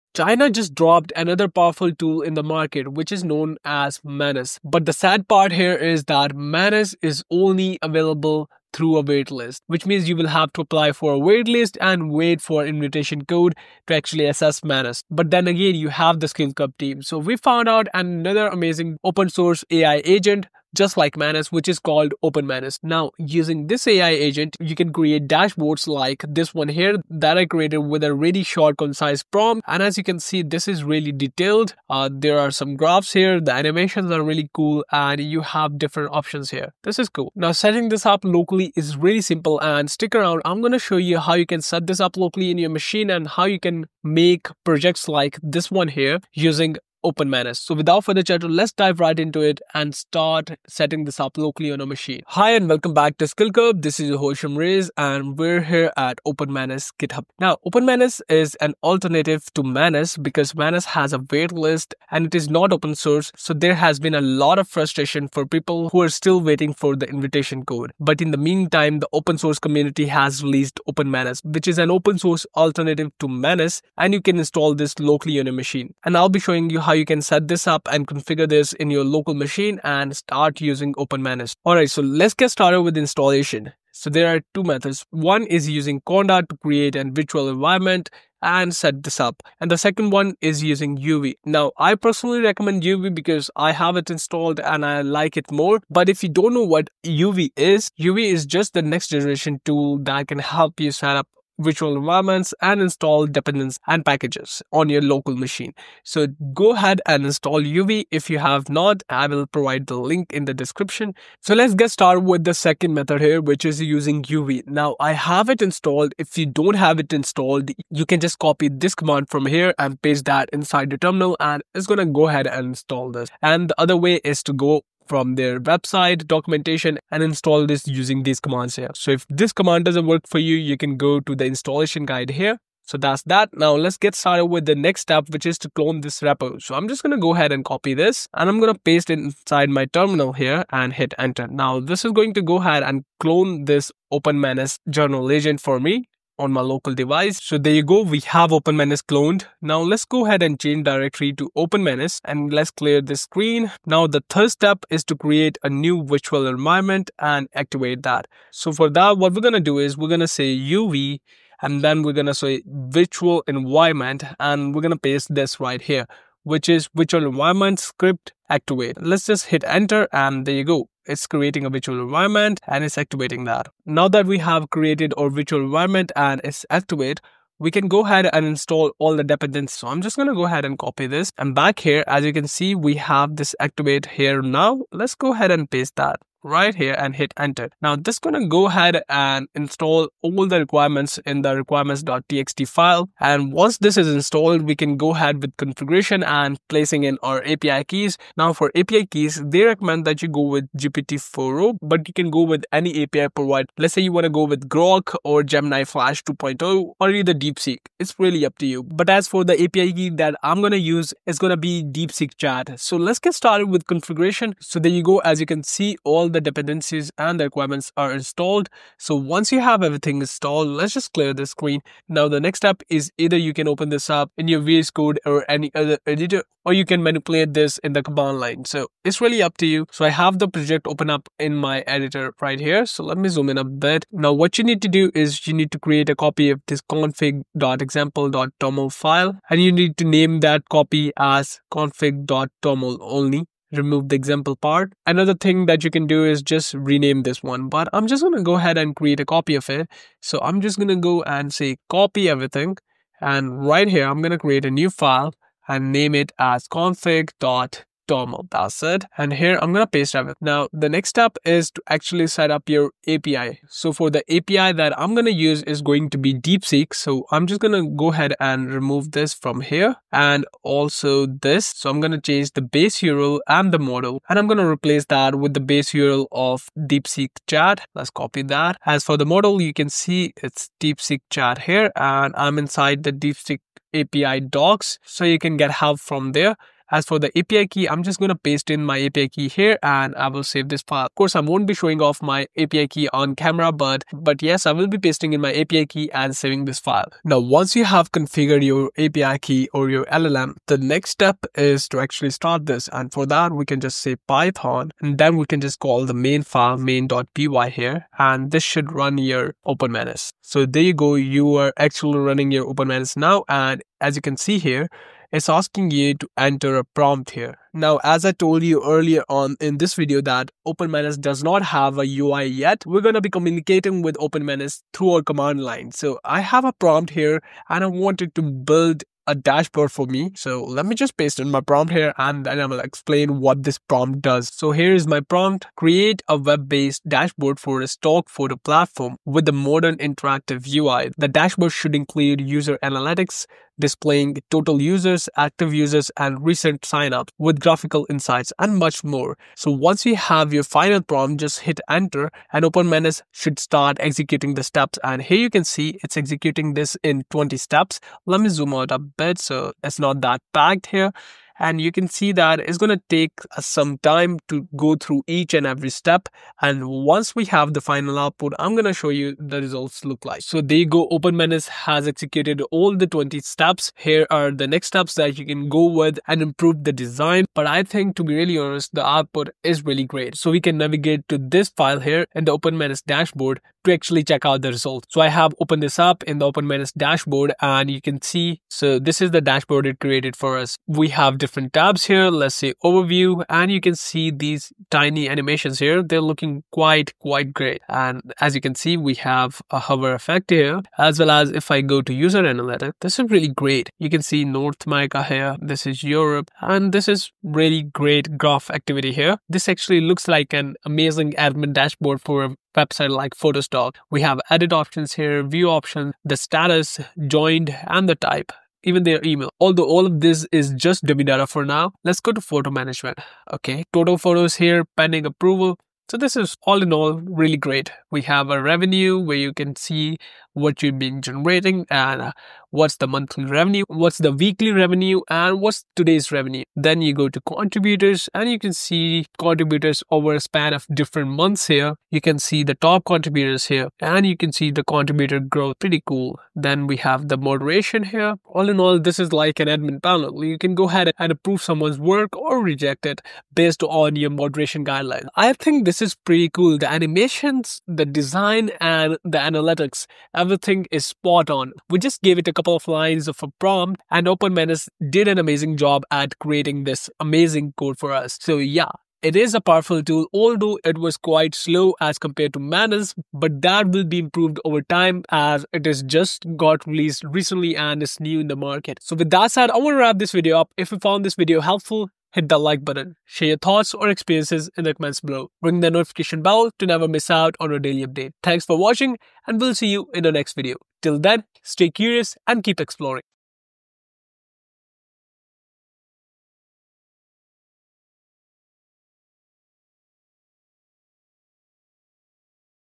OpenManus – The Best Alternative to Manus AI Agent! 🚀 (Local Install)
Want to set up OpenManus, a powerful alternative to Manus AI Agent? In this video, we’ll guide you through the installation and setup process of OpenManus, helping you get started with this open-source AI agent.

🔹 Video Timeline:
 00:00 – Introduction
 00:52 – Setting up OpenManus
 05:10 – Testing OpenManus
 07:35 – Final Thoughts

Top AI Image & Video & Audio tools we Recommend

Digtal Assets: 🔥🔥 Get Envato's Unlimited Download for Royalty Music & Video library